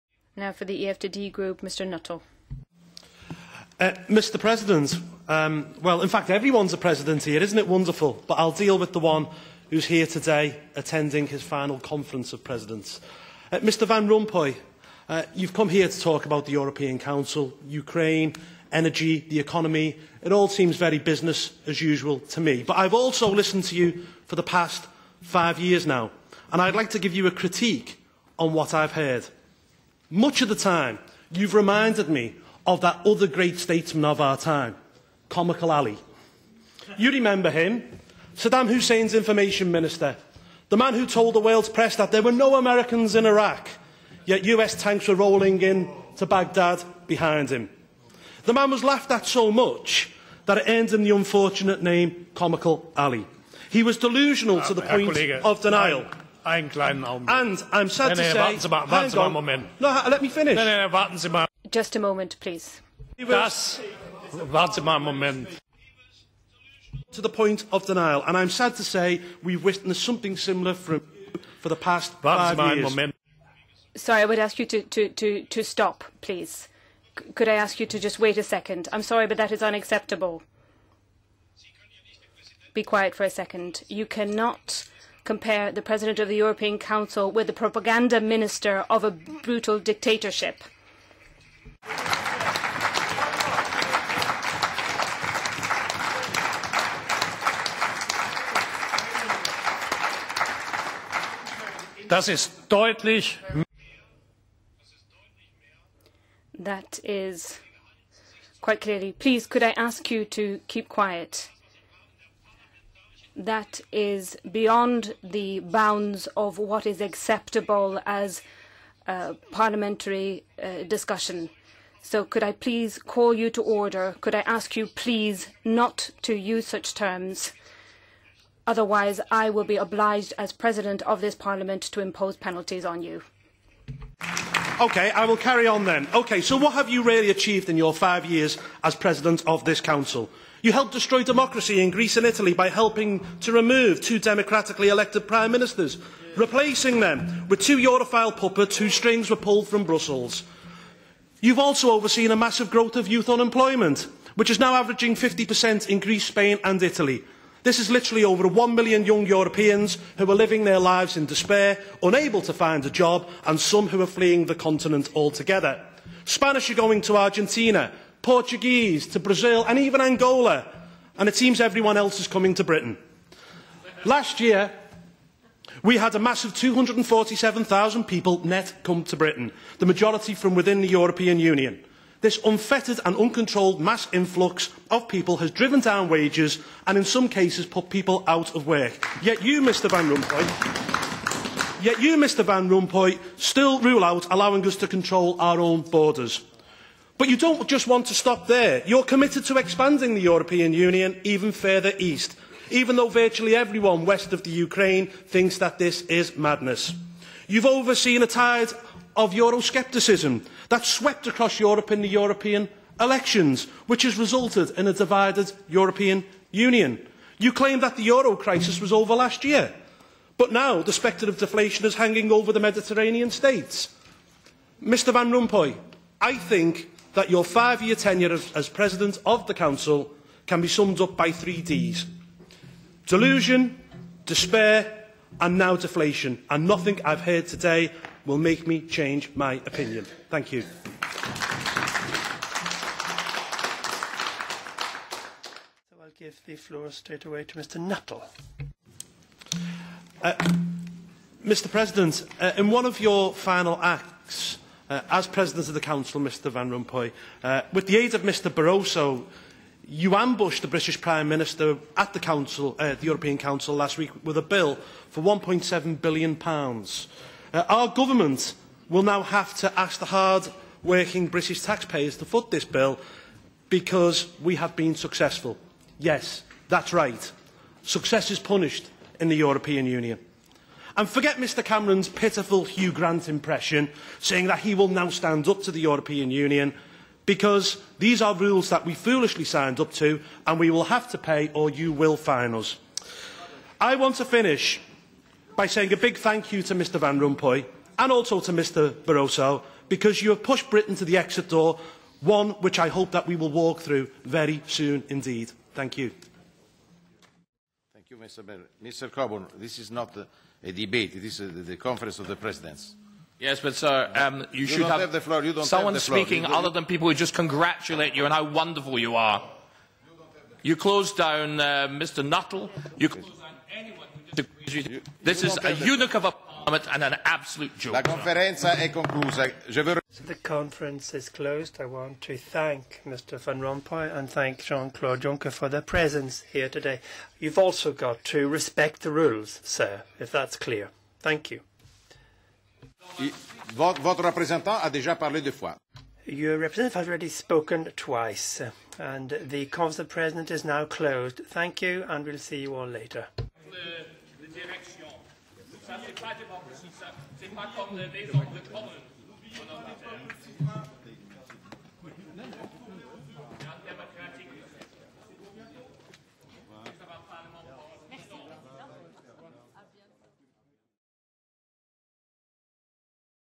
Paul Nuttall: You remind me of Comical Ali, Mr Van Rompuy (includes two speeches by Mr Nuttall)
•  European Parliament, Brussels, 04 November 2014

• Paul Nuttall MEP, Deputy Leader, UK Independence Party (North West), Europe of Freedom and Democracy (EFD) group

• David COBURN MEP UK Independence Party (Scotland), Europe of Freedom and Direct Democracy (EFDD) group - @DavidCoburnUkip

• Conference of Presidents - with outgoing EU Council president Herman van Rompuy

Transcript:

Mr President, well, in fact everyone's a president here, isn't it wonderful! But I'll deal with the one who is here today attending his final conference of presidents.
Mr Van Rompuy, you've come here to talk about the European council, Ukraine, energy, the economy - it all seems very business-as-usual to me. But I've also listened to you for the past five years now and I'd like to give you a critique on what I've heard.

Much of the time you've reminded me of that other great statesman of our time, Comical Ali. You remember him, Saddam Hussein's information minister. The man who told the world's press that there were no Americans in Iraq yet US tanks were rolling into Baghdad behind him. The man was laughed at so much that it earned him the unfortunate name, Comical Ali. He was delusional to the point of denial... [European Parliament President Schulz interferes]

Martin Schulz: Just a moment, please. Sorry, I will ask you to stop, please, wait a moment. This is unacceptable, be quiet. You cannot compare the President of the European Council to the propaganda minister of a brutal dictatorship.

That is beyond the bounds of what is acceptable as parliamentary discussion so could I please call you to order, and ask you to not use such terms, otherwise I will be obliged to impose penalties on you.

Paul Nuttall: Okay, I will carry on then. So what have you really achieved in your five years as president of this council? You helped destroy democracy in Greece and in Italy by helping to remove two democratically elected prime ministers replacing them with two europhile puppets whose strings were pulled from Brussels. You've also received a massive growth in youth unemployment which is now averaging 50 percent in Greece, Spain and Italy. This is literally over 1 million young Europeans who are living their lives in despair, unable to find a job and some who are fleeing the continent altogether. Spanish are going to Argentina, Portuguese to Brazil and even Angola! And it seems everyone else is coming to Britain. Last year we had a massive 247 million people net come to Britain, the majority from within the EU. This unfettered and uncontrolled mass influx of people has driven down wages and in some cases put people out of work. Yet you Mr Van Rompuy still rule out allowing us to control our own borders. But you don't just want to stop there, you are committed to expanding the EU even further east, even though virtually everyone west of the Ukraine thinks that this is madness. You've overseen the tide of euroscepticism that has swept across Europe in the European elections which has resulted in a divided European Union, deflation hanging over the Mediterranean states. Mr Van Rompuy, I think that your five year tenancy as president of the European Council can be summed up with three Ds. Delusion, despair and deflation and nothing I've heard today will make me change my opinion.

Speech TWO

In one of your final acts as president of the council Mr Van Rompuy, with the aid of Mr Barroso, you ambushed the David Cameron the Prime Minister at the council last week with a bill for 1.7 billion pounds. Our government will now have to ask the hard working British tax payers to foot this bill because we have been successful. Yes that's right, success is punished here in the EU and forget Cameron's pitiful Hugh Grant impression saying that he will now stand up to the EU, because these are rules that we foolishly signed up to and we will have to pay or you will fine us. I want to finish by saying a big thank you to Mr Van Rompuy and also to Mr Barroso because you have pushed Britain to the exit door, one which I hope that we will walk through very soon indeed. Thanks.